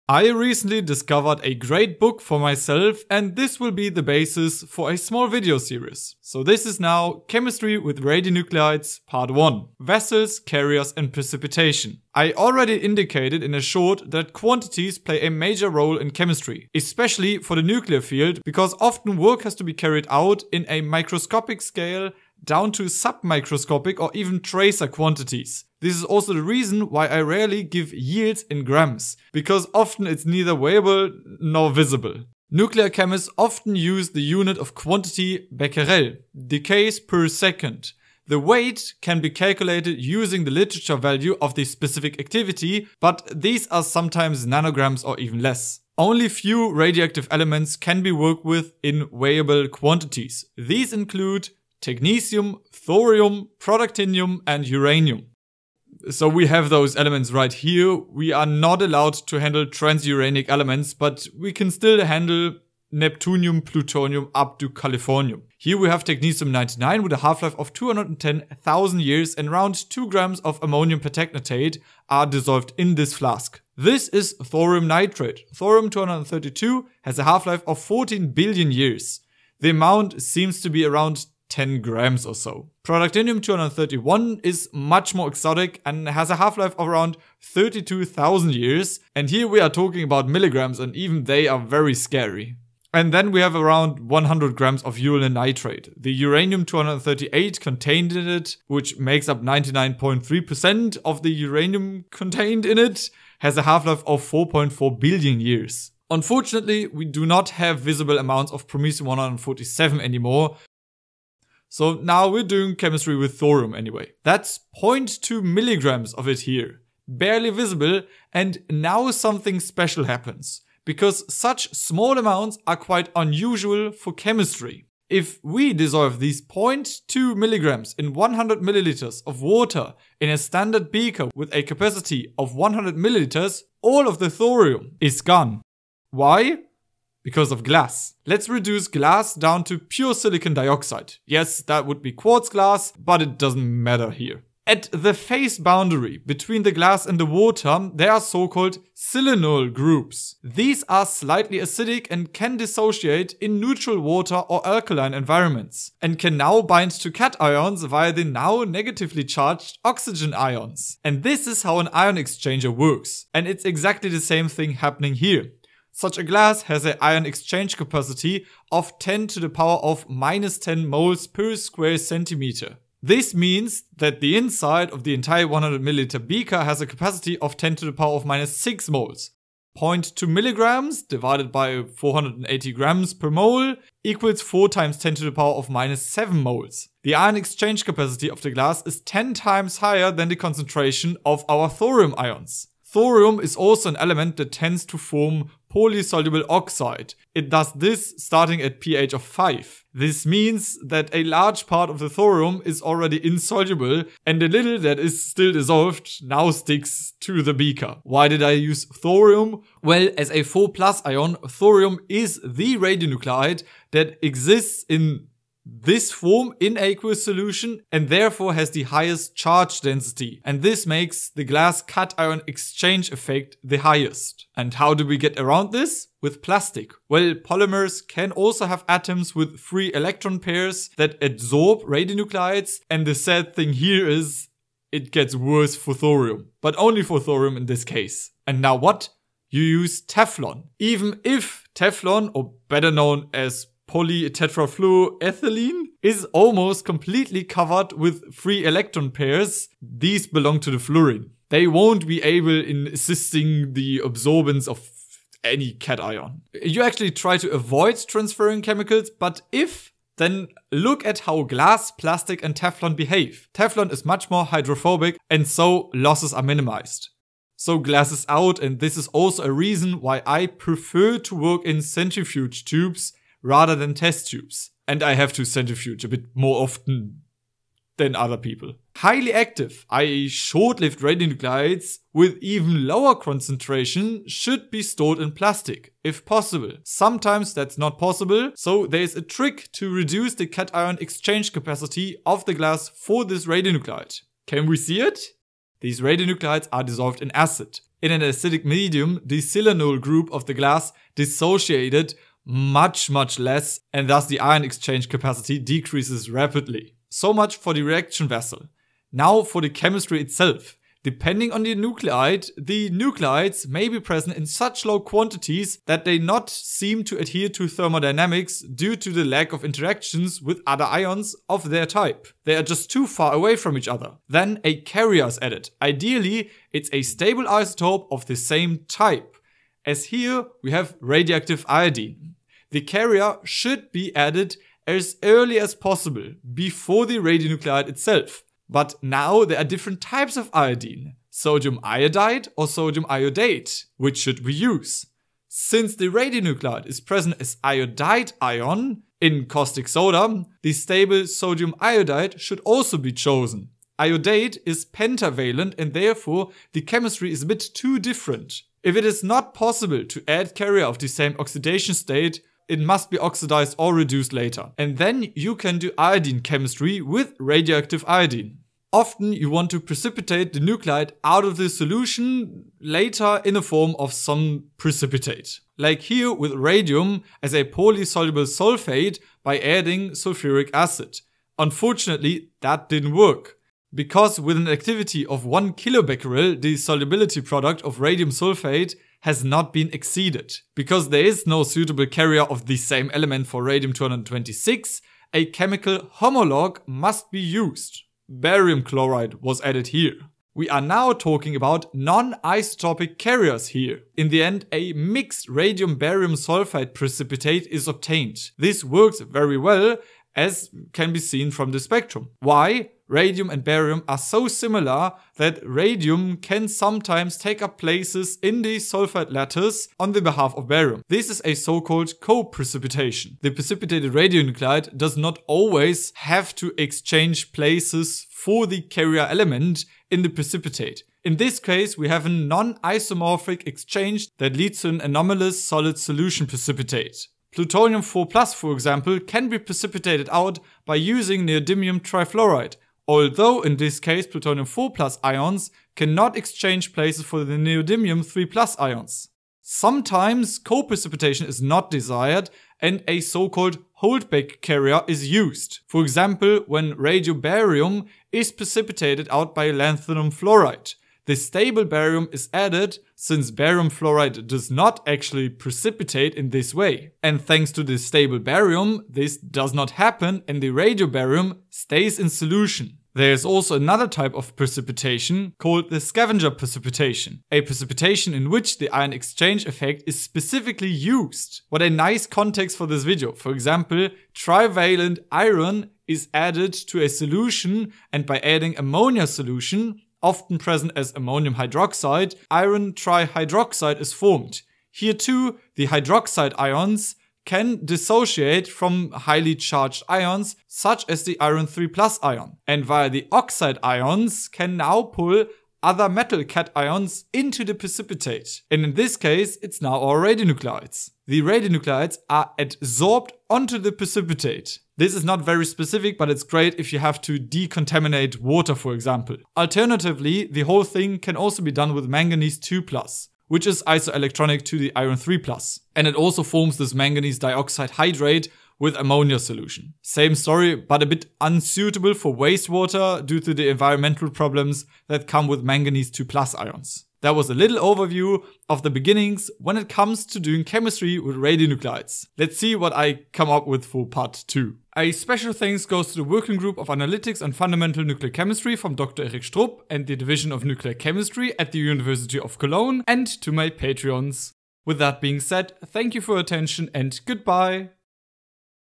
Chemistry with Radionuclides Pt.1: Vessels, Carriers, Precipitations
Chemistry and Analysis of Radionuclides: Laboratory Techniques and Methodology
Author(s):Prof. Jukka Lehto, Dr. Xiaolin Hou
First published:24 November 2010
Print ISBN:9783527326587 |Online ISBN:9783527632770 |DOI:10.1002/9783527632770

PXRD: RaBaSO4
Matyskin, A. V., Ylmen, R., Lagerkvist, P., Ramebäck, H., & Ekberg, C. (2017). Crystal structure of radium sulfate: An X-ray powder diffraction and density functional theory study. Journal of Solid State Chemistry, 253, 15–20.

PXRD: NdF3
Neutron diffraction study on NdF3 by
Kondratyuk, I.P.;  Loshmanov, A.A.;  Muradyan, L.A.;  Maksimov, B.A.;  Sirota, M.I.;  Krivandina, E.A.;  Sobolev, B.P.;

0:00 Hello :)
0:51 carrier free elements
1:50 vessels
5:08 isotopic carrier
6:07 non-isotopic carrier
7:29 Precipitates
9:05 bye :)